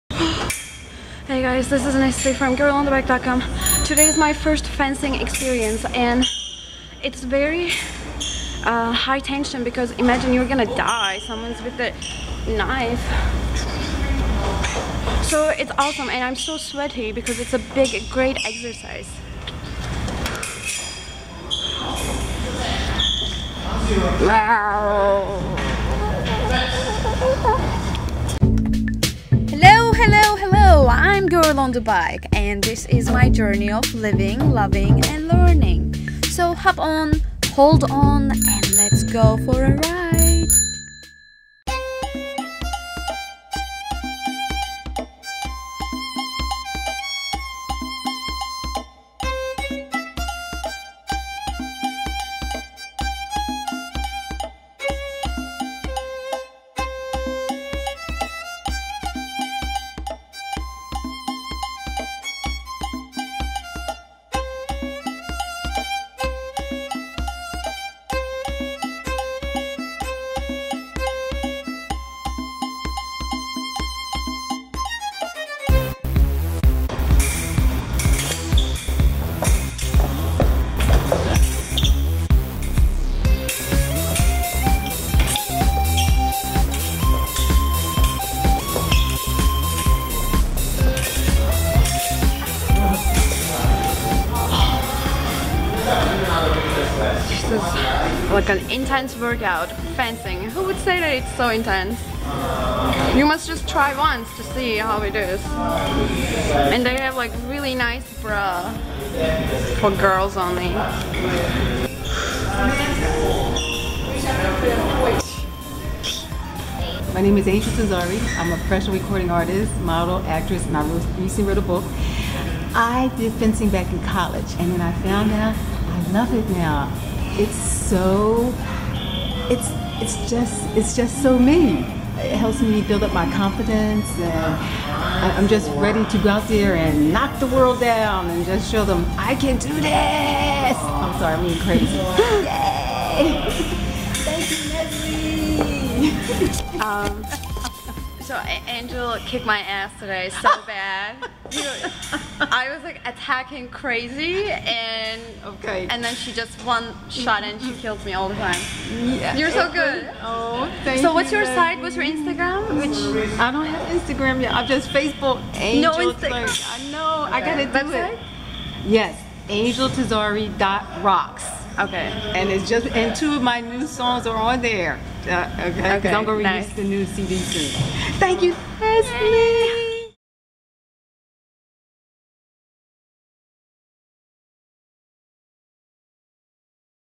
Fencing FIRST TIME! Manhattan Fencing Center
This week, I'm sharing my first fencing experience, I had no idea what was waiting for me, again! (e.g hiking video)